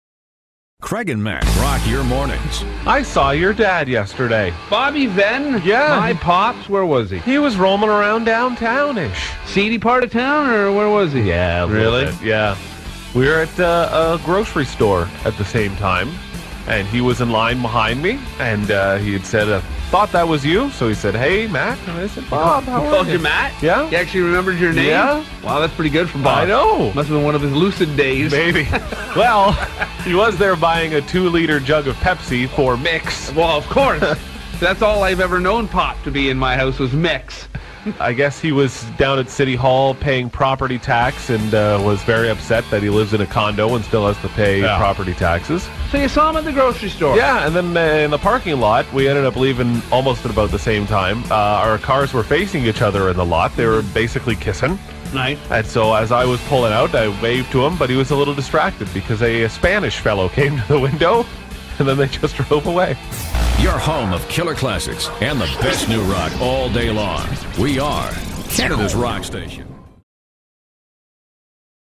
Spend a Minute With Craig & Matt- I Saw Your Dad
Its a small world Craig and Matt run into each other's family, and their dads get scared of spanish people coming to window..